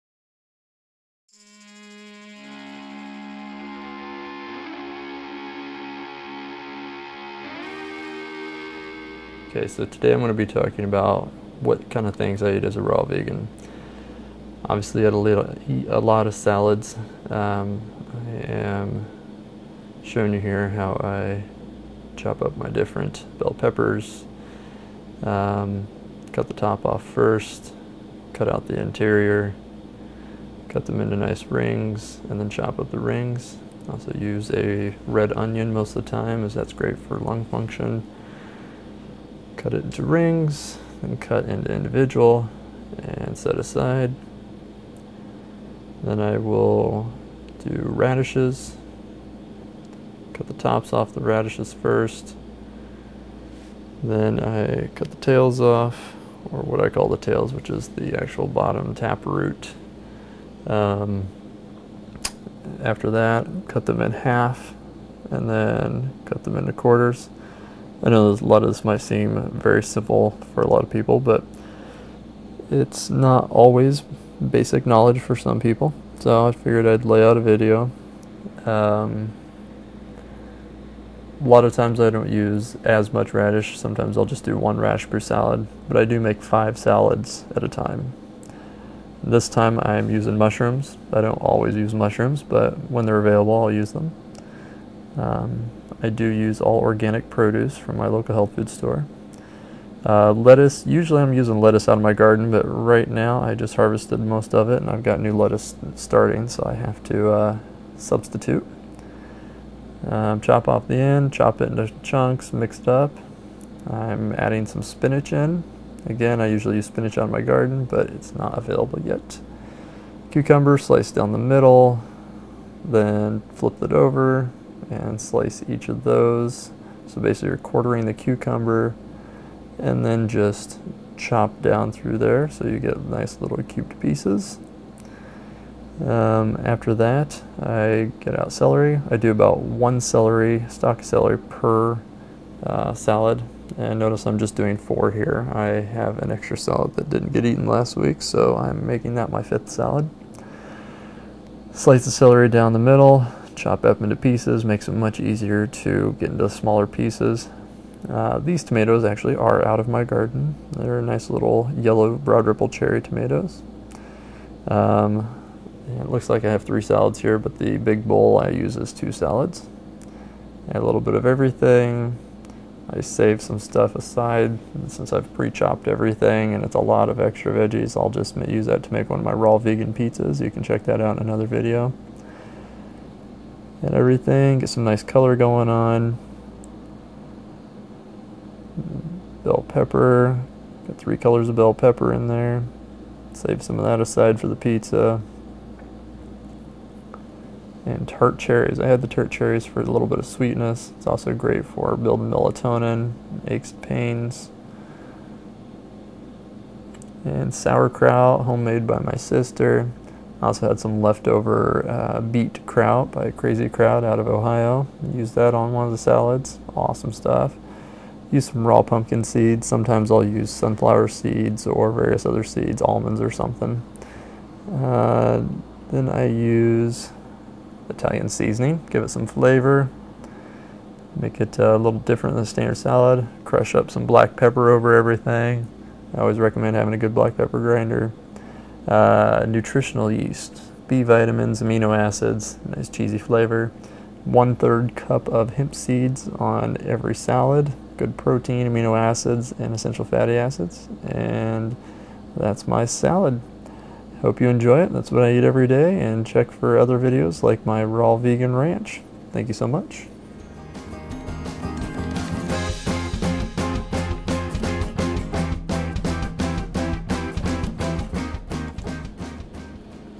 What I Eat As A Raw Vegan - Salads
What I Eat #1 - A lot of people ask me about what I eat since I'm Raw Vegan, so I've decided to start making some videos. This first video is showing how I make my salads, what I put into them, and why. Hope you enjoy!